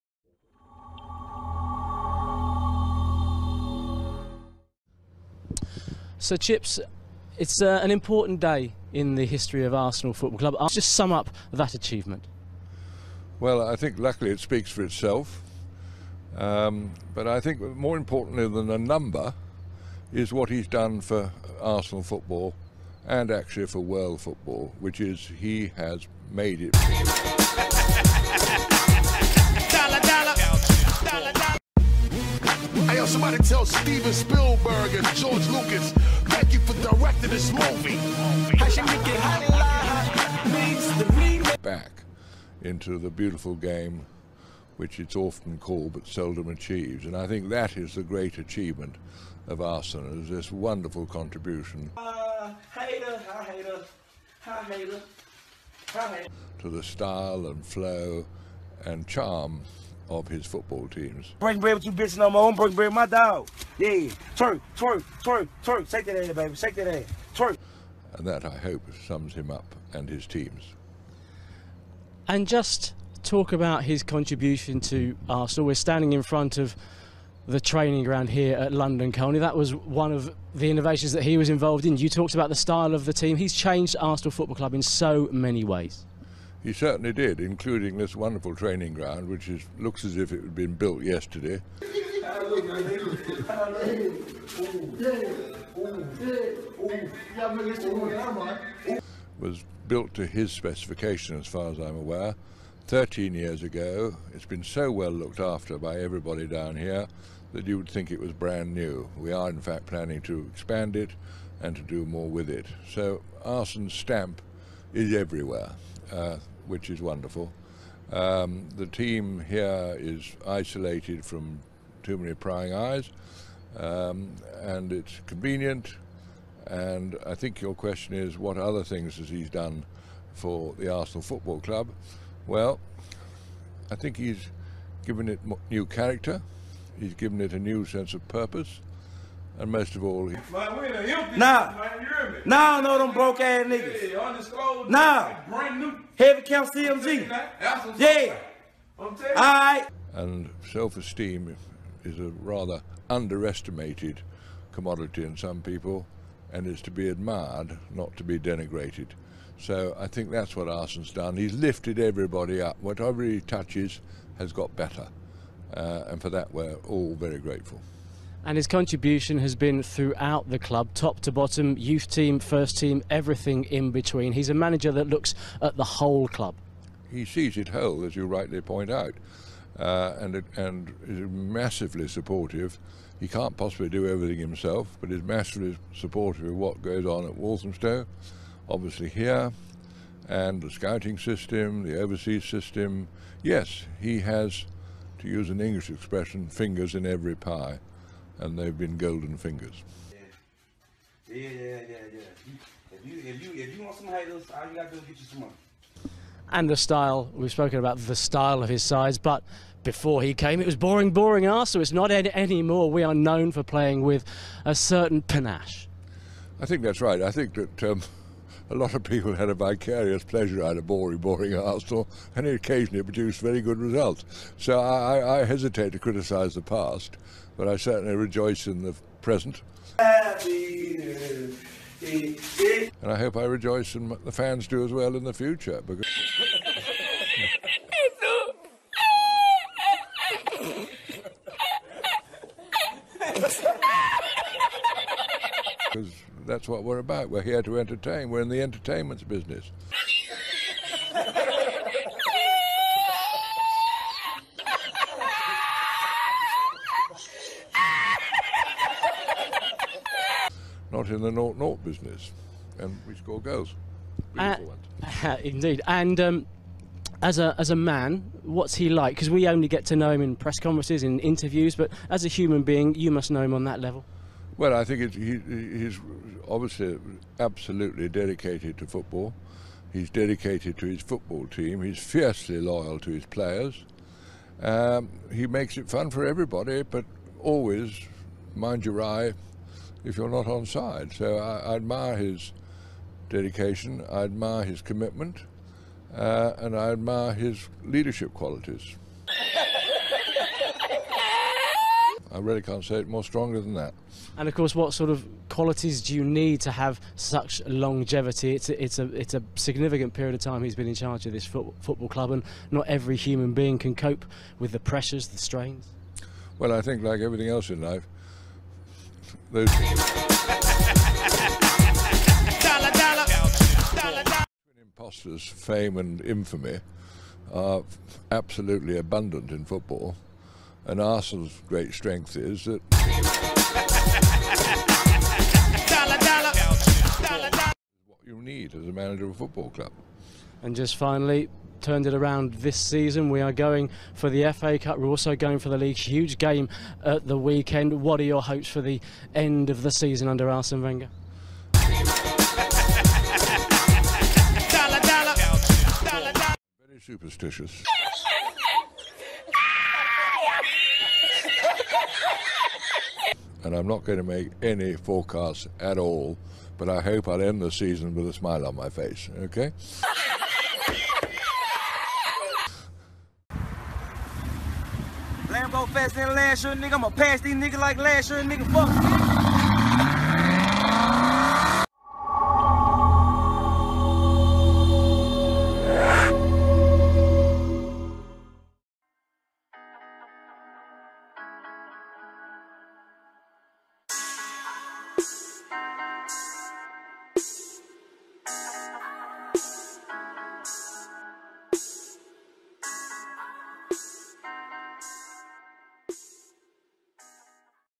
Sir Chips Keswick After AGM 2017 EXCLUSIVE INTERVIEW (Parody)
Exclusive interview from Sir Chips Keswick after the poorly managed AGM this year of 2017. Here he goes in depth about Arsene Wenger and Arsenal football club.